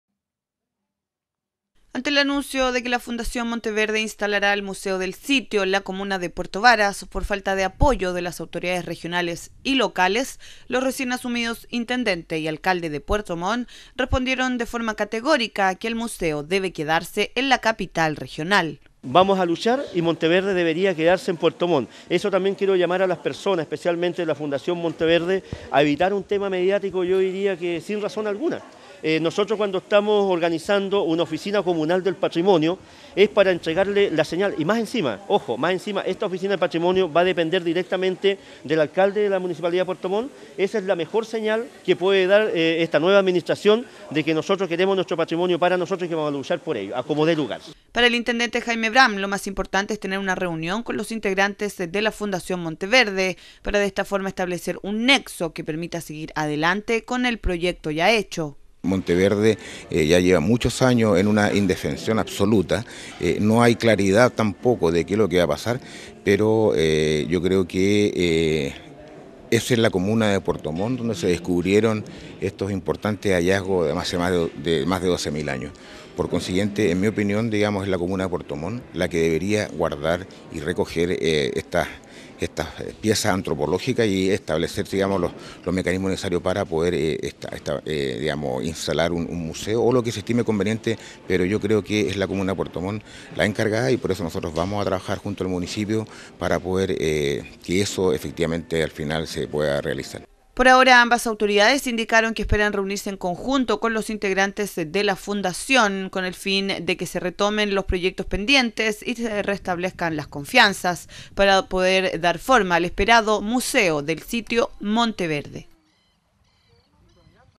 Autoridades piden museo de Monteverde
Los recién asumidos, Intendente de la Región de los Lagos y el Alcalde de Puerto Montt señalaron que quieren el museo Monteverde en la capital regional.